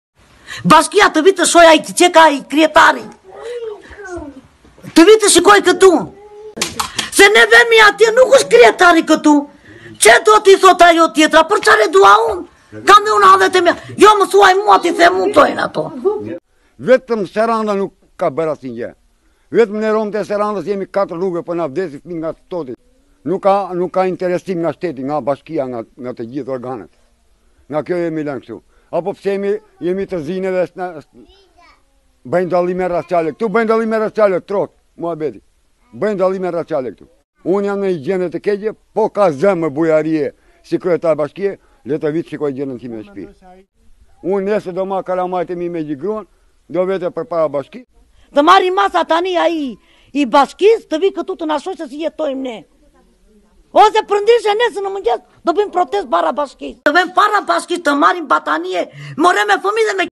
Romët e Çukës: Bashkia Sarandë bën dallime raciale
Të lodhur nga pritja, premtimet dhe mashtrimet, ata kërcënojnë se do të kalojnë në veprime ekstreme